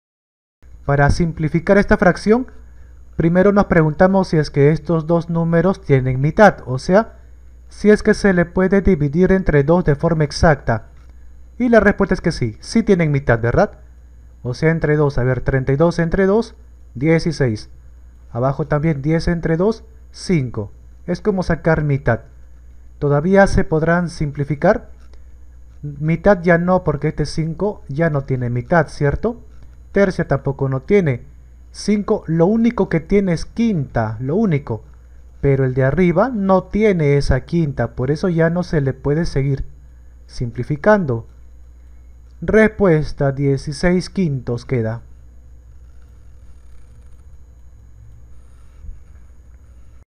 simplificar 32/10 , simplificar fracciones a su expresion mas simple
simplificar fracciones
reducir fracciones a su expresion mas simple